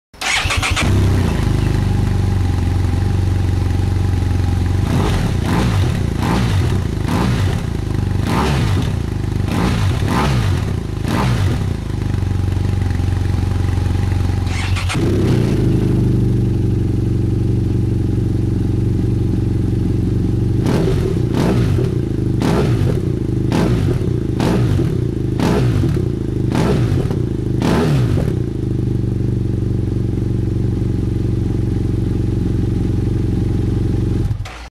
BMW F800GS Leo Vince Evo II (with and without baffle)
Here is a sound clip of my Leo Vince Evo II slip-on with and without the baffle on my F800GS.